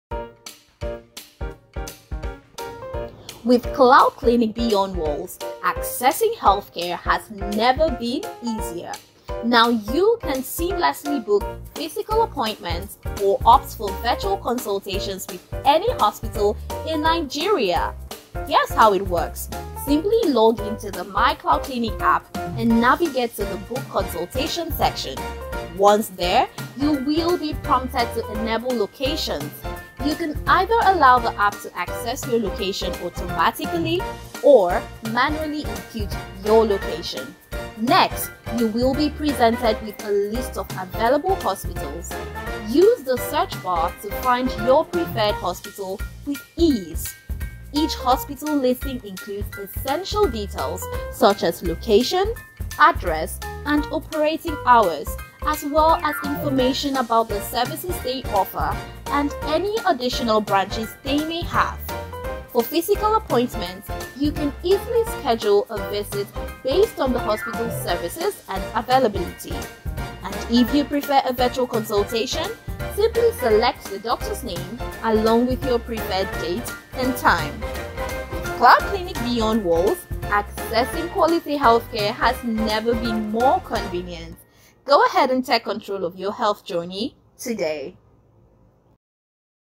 Beyond the Walls: Exploring the Evolution of Hospital Services!
We’re breaking barriers, taking hospital services beyond the confines of physical walls. Now, hospitals can reach their patients anytime, anywhere.

Imagine your doctors connecting with patients far and wide, saving lives without limitations. That’s the power of virtual connections with BeyondWalls.